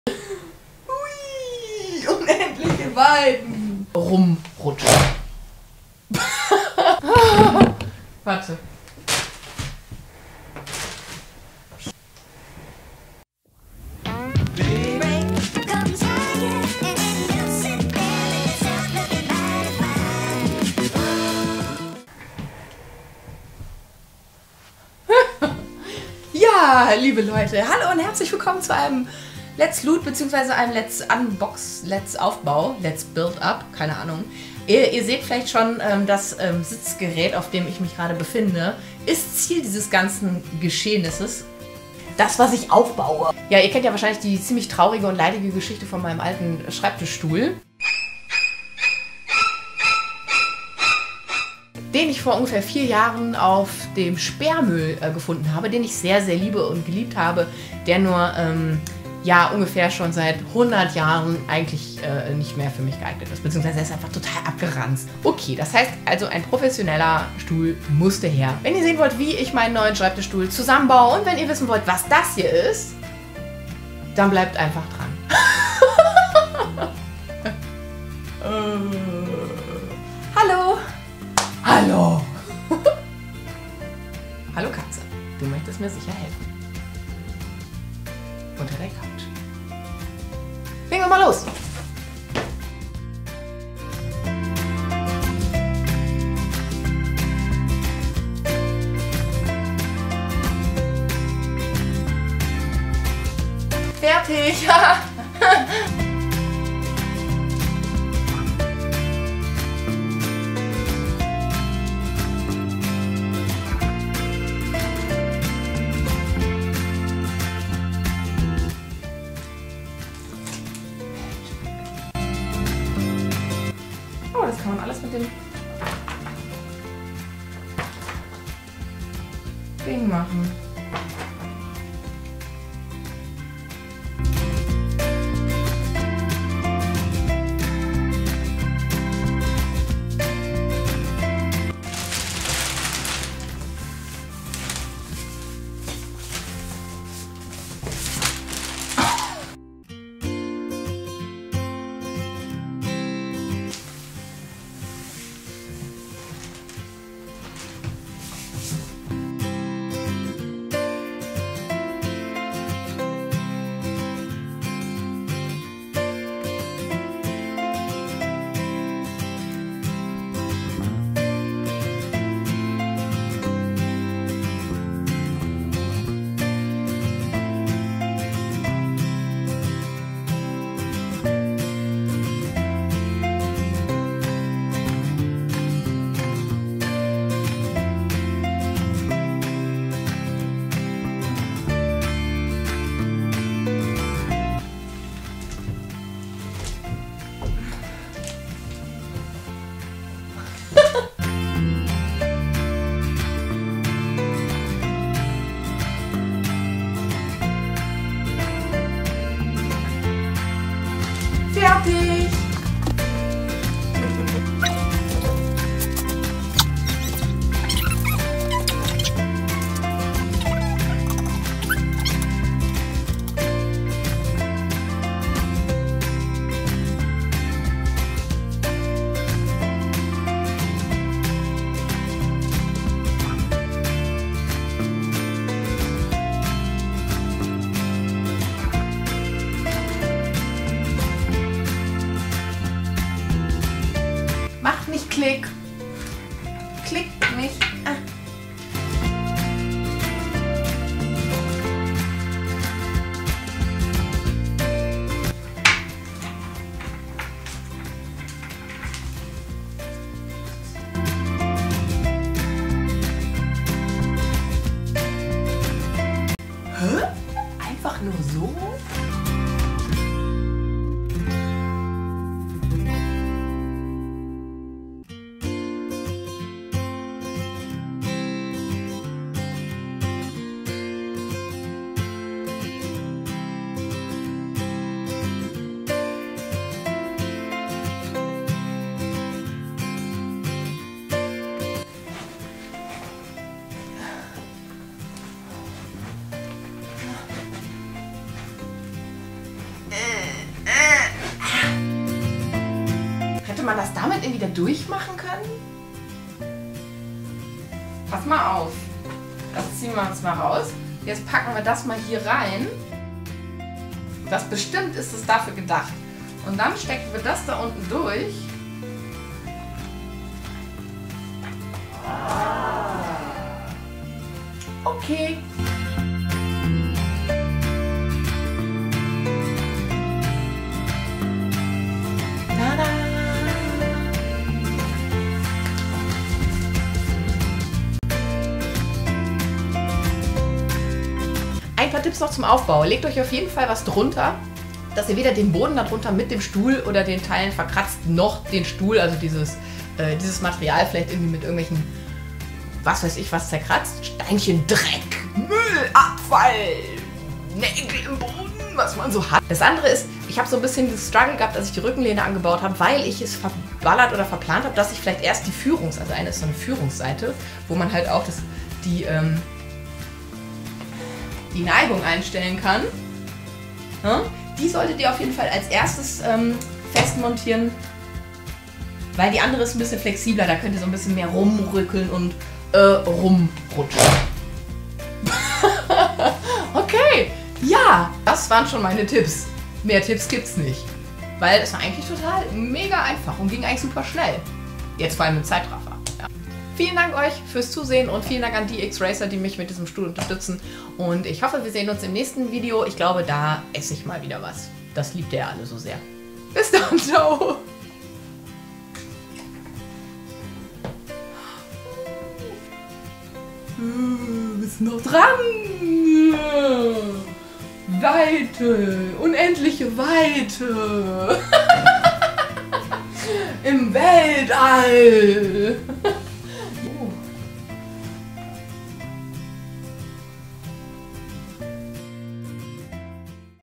Let's Build » knallhart recherchiert » Ive packt aus » DXRacer F-Series aufbauen
» Let's Build - DXRacer F-Serie
» Ive unterstützen? Einfach über diesen Link shoppen

Die Qualen haben endlich ein Ende, Rückenschmerzen gehören der Vergangenheit an und ich muss nie wieder aufstehen!
Seid dabei, wenn ich meinen neuen Gaming Stuhl auspacke und aufbaue!

Vielen Dank an für die kostenlose Bereitstellung des komfortablen Sitzgerätes!

Gefilmt mit der Canon 70D